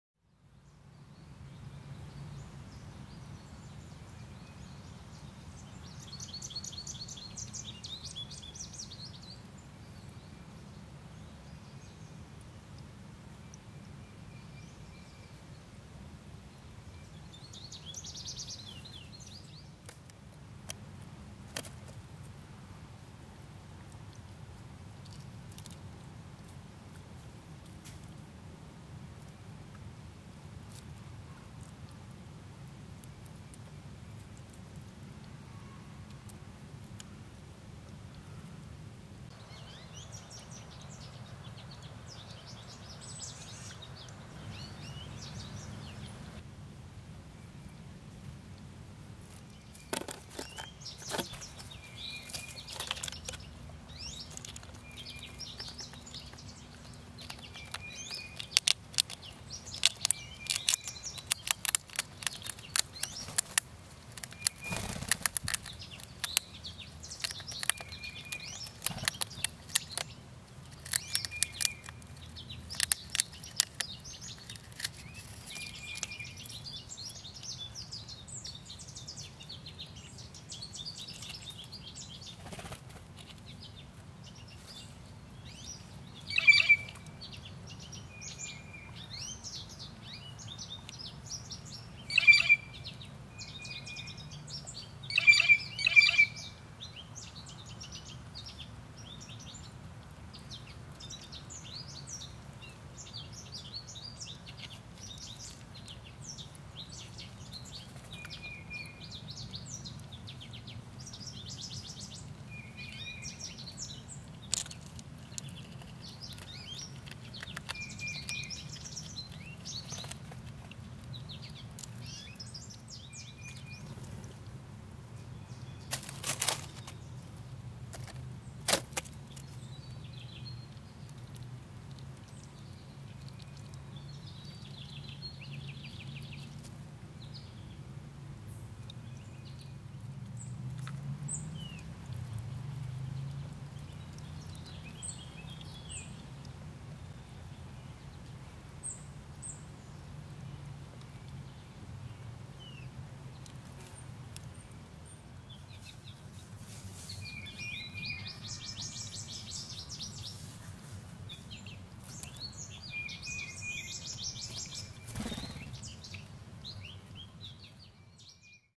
Lil Sleek (Natural Sounds)
A simple fixed camera shot of  Lil Sleek the chipmunk eating with a scenic backdrop. At one point there were 3 blue jays above him. You can hear them calling. Only one bird was brave enough to have lunch at the bottom of the falls:)